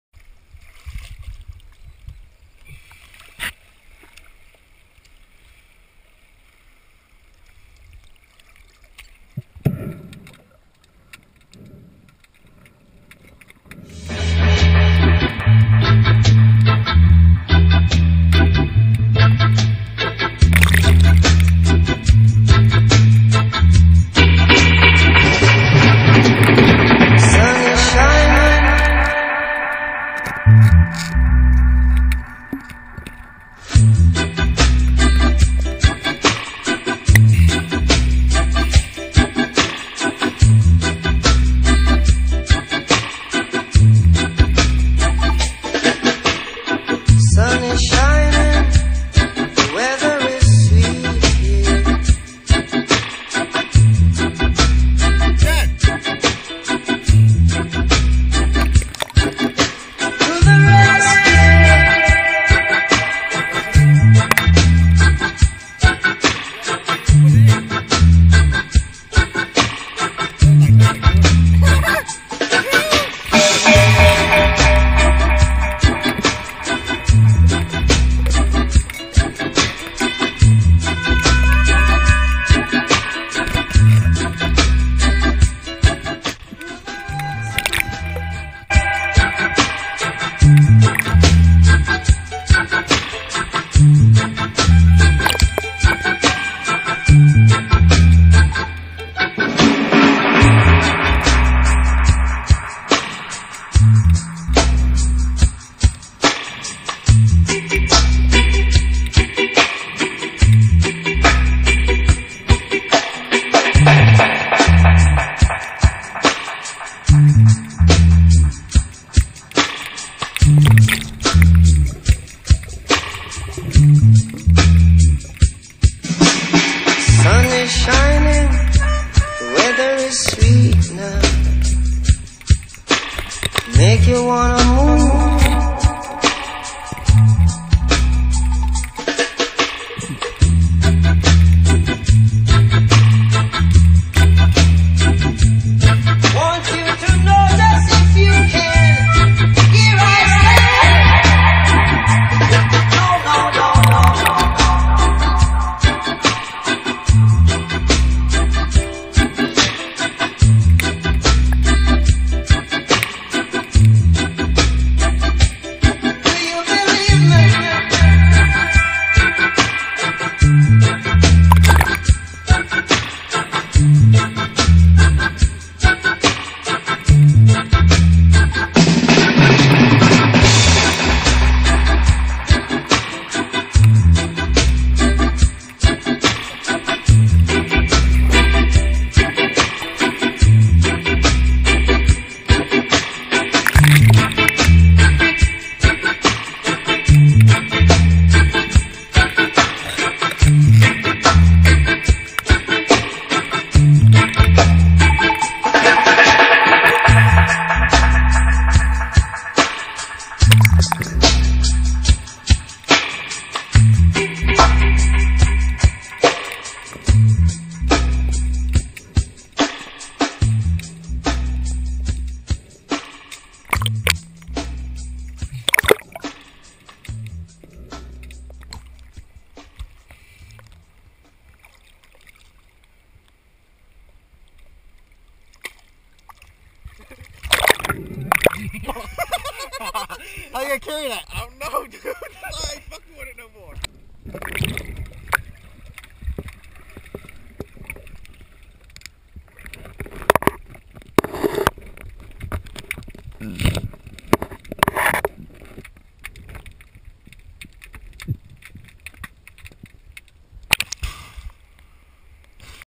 Abalone diving
5-12-13
Went to the north coast to dive for some abalone. i went with my dad and my buddy john. my dad wasnt in the video but my buddy john had a bag full of big sea urchins it was a great day to be in the water